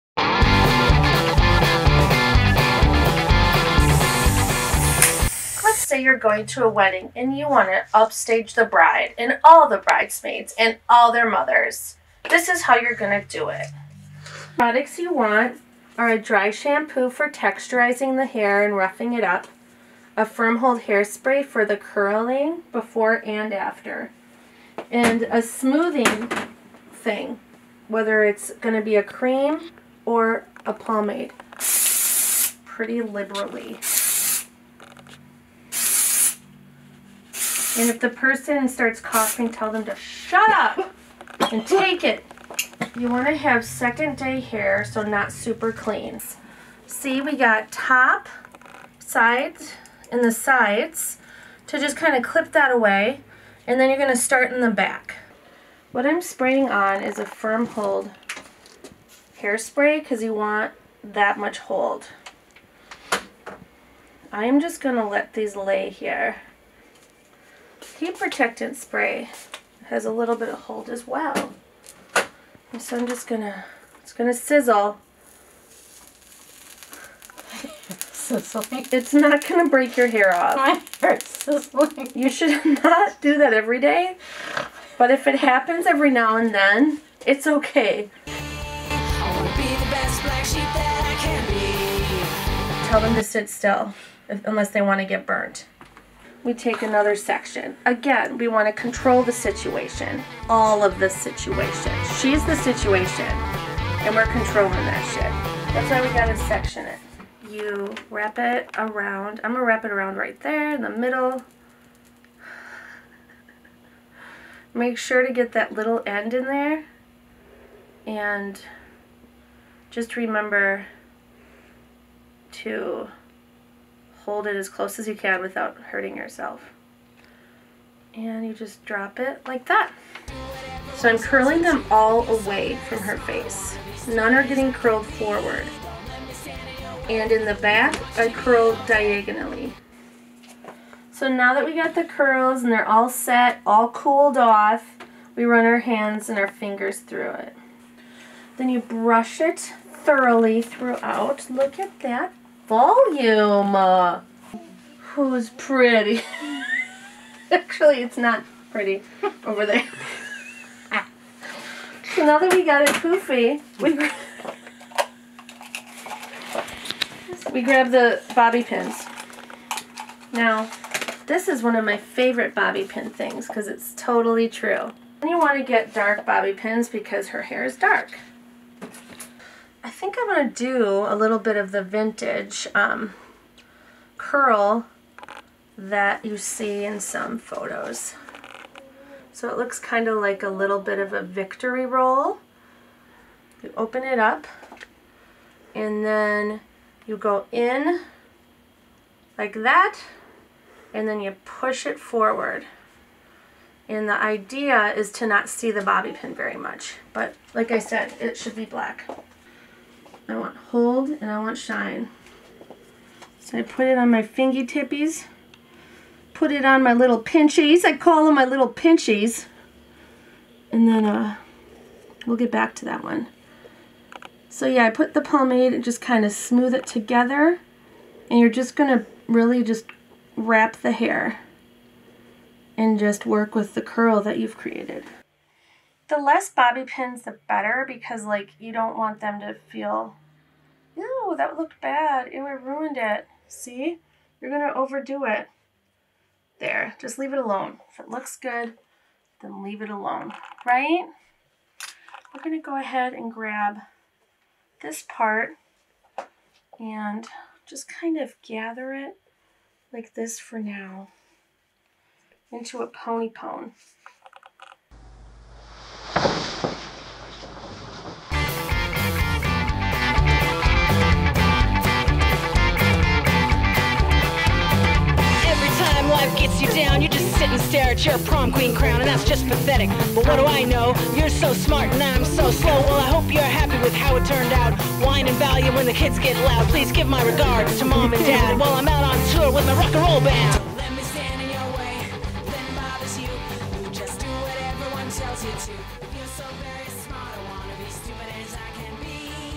UpDo creds
Sarah Hesse Shows how to do hot curls and an updo!